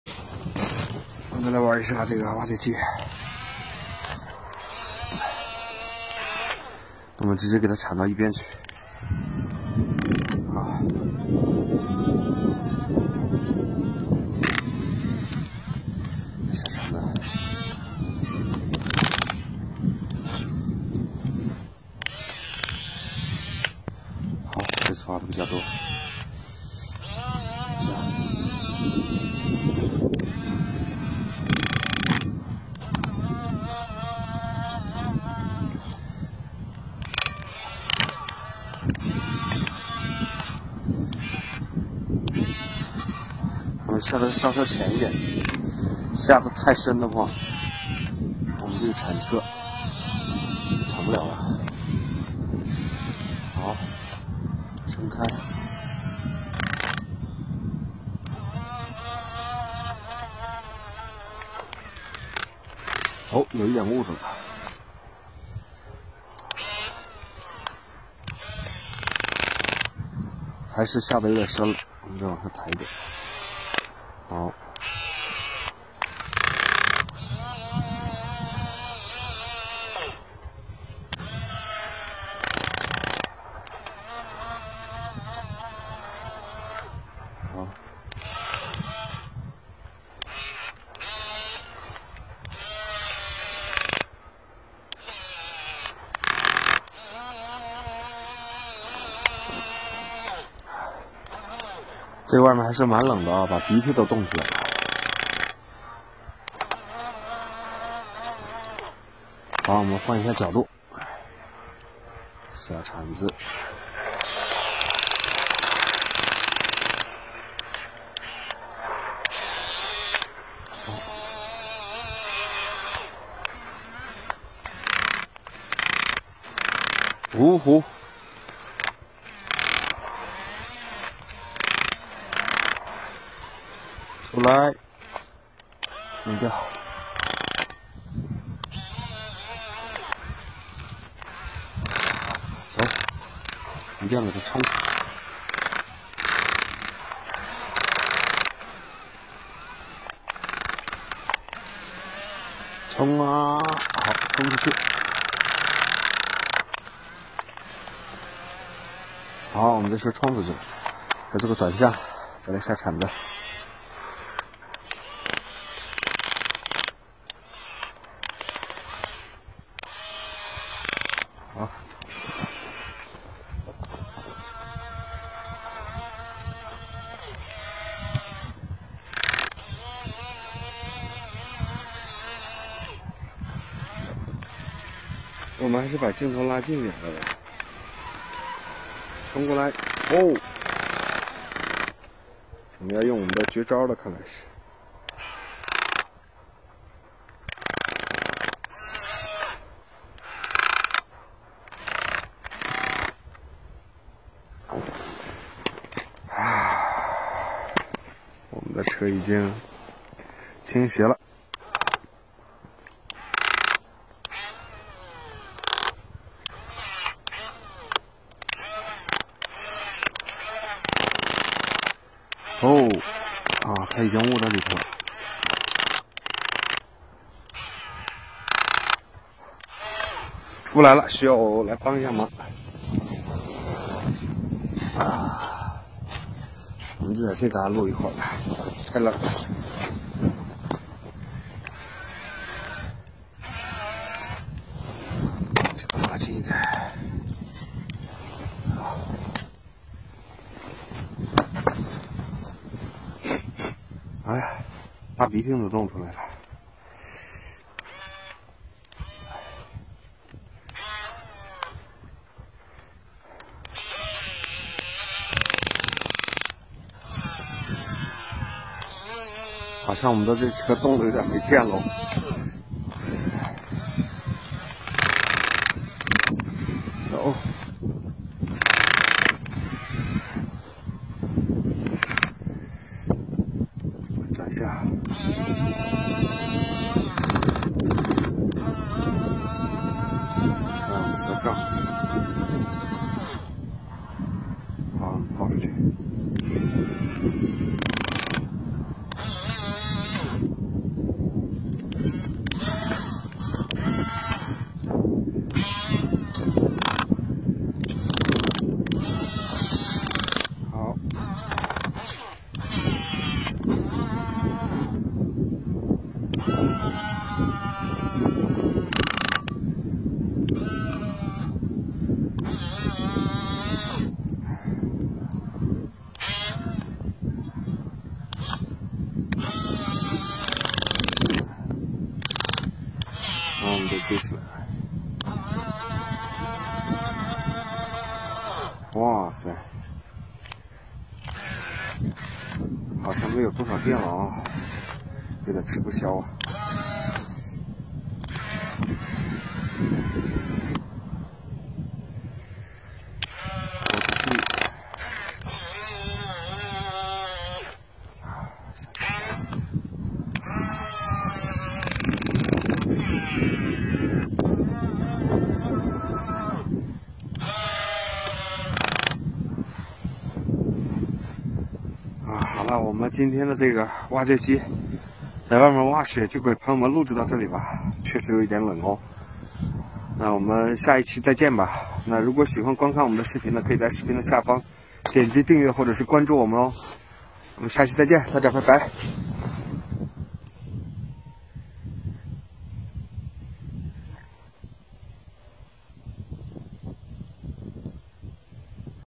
RC Toy Evcavator 2 4G HZ Working In Cold Winter 2
Warm Family Playing RC Toy Evcavator Enjoy In Cold Winter